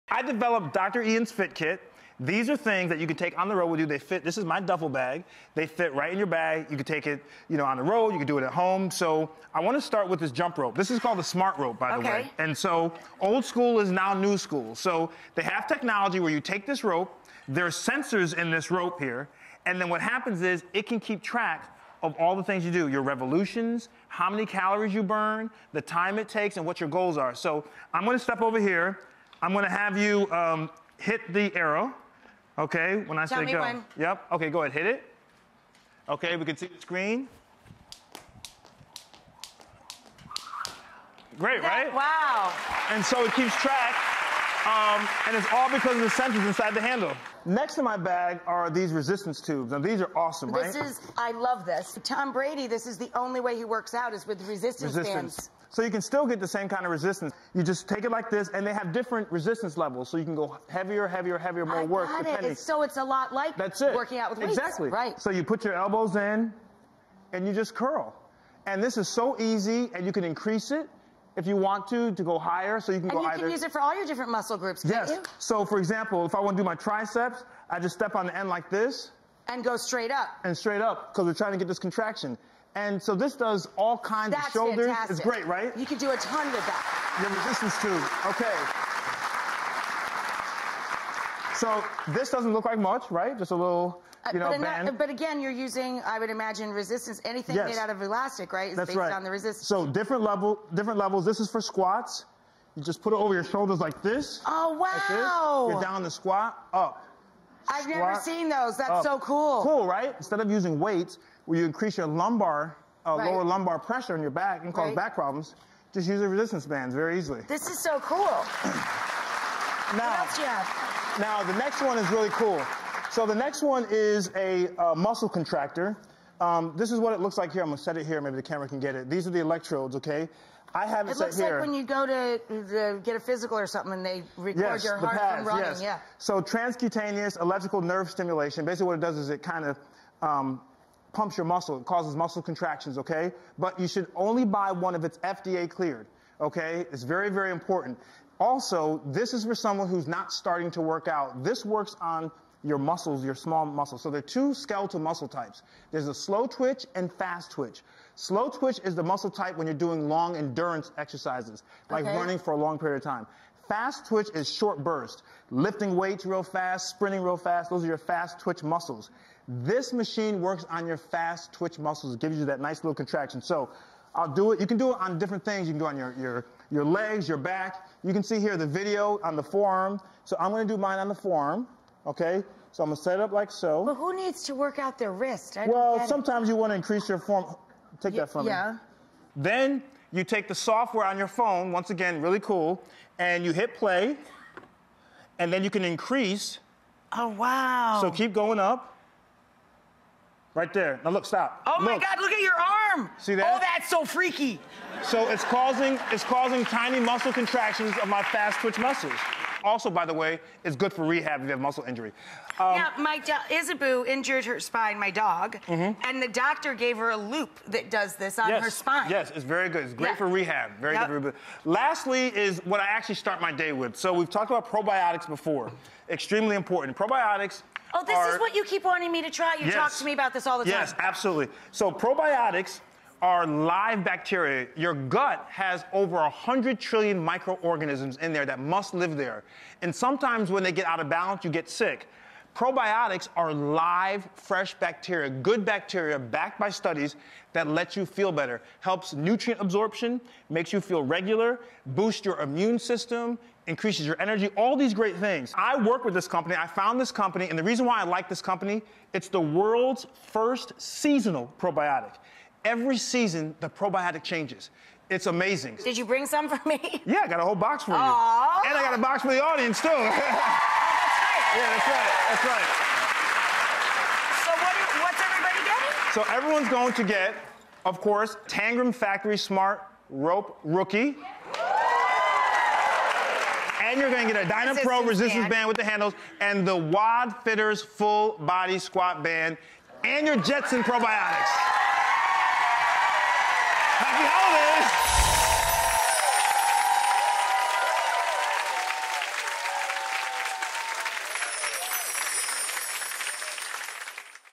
Travel Workout Kit | Dr. Ian Smith
Physician Dr. Ian Smith shares the items he uses to exercise while traveling — when he doesn't have an elliptical or weights.